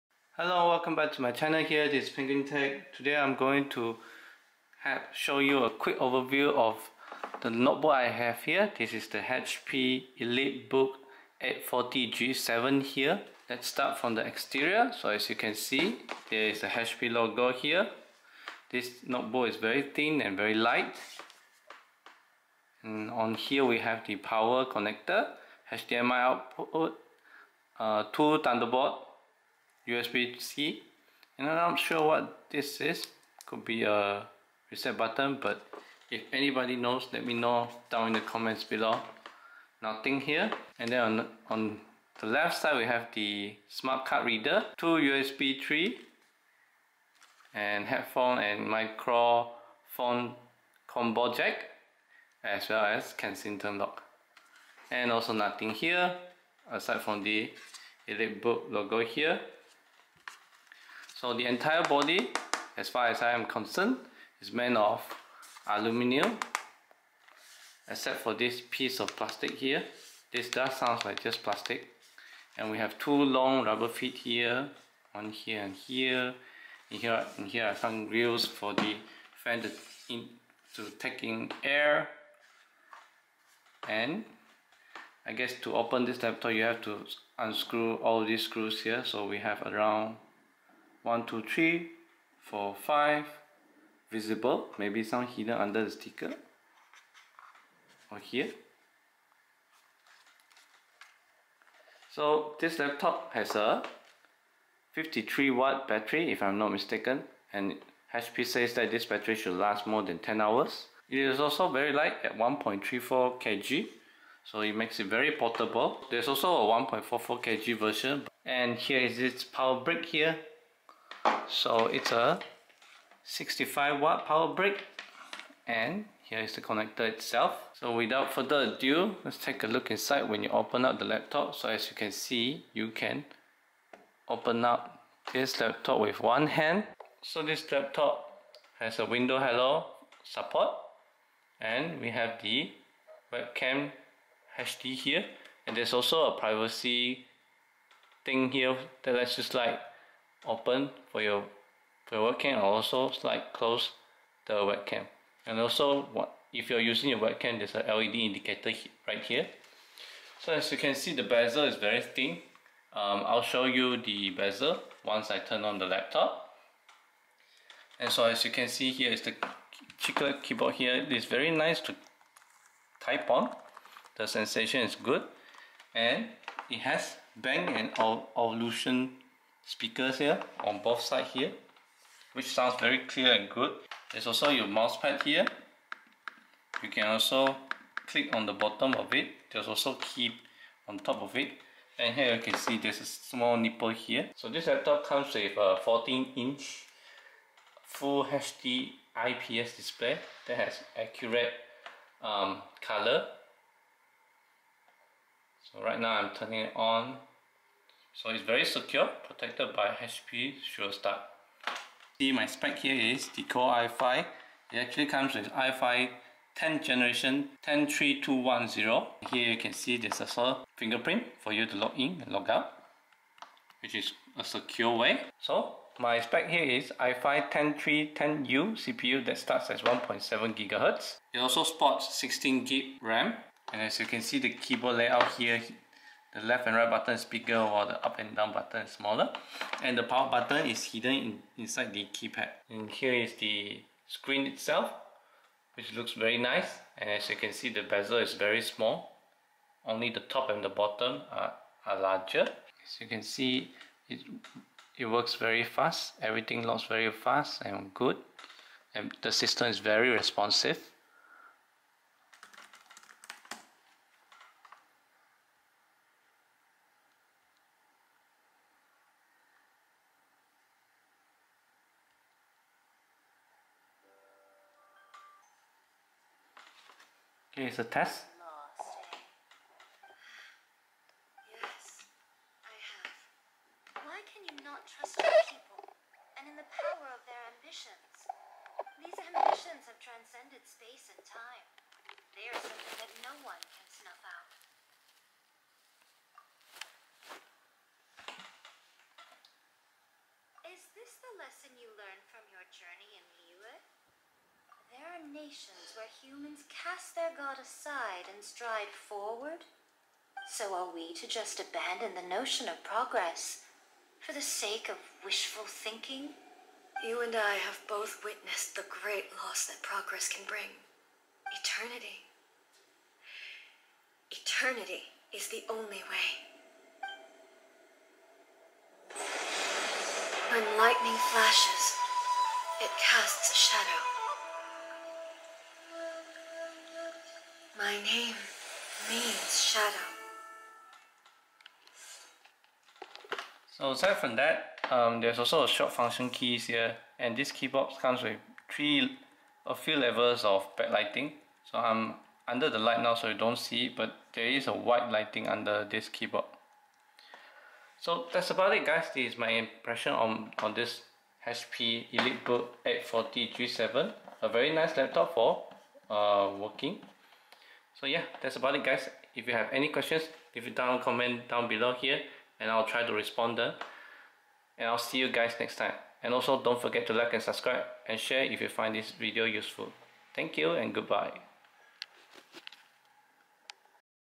QUICK OVERVIEW - HP ELITEBOOK 840 G7 (I5 10TH GENERATION)
Quick overview of HP Elitebook 10th Gen which I use as my daily driver for work. Drop your comments below and I will try my best to reply them!